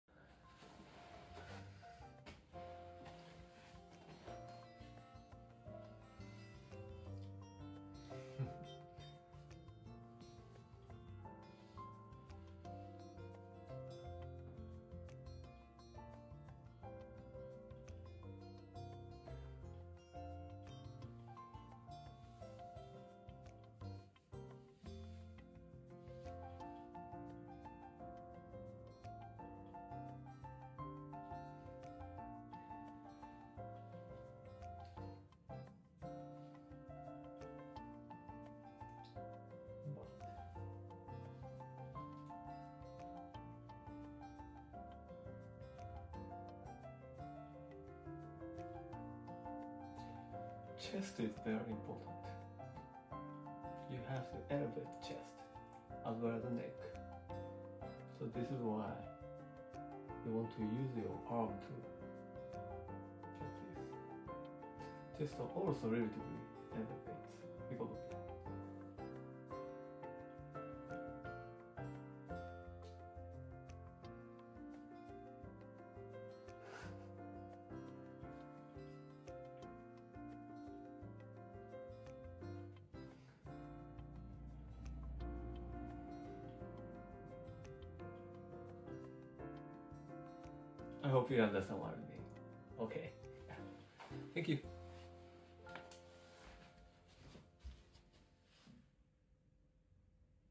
Quick Correction for Slouched Posture While Sitting by Tomoya
Quick but effective tip for slouched posture and lower back pain on a chair.
Tomoya Kondo
Conditioning Trainer, Master with Kinesiology(thesis in progress)(USA)
Have run the business(Japan)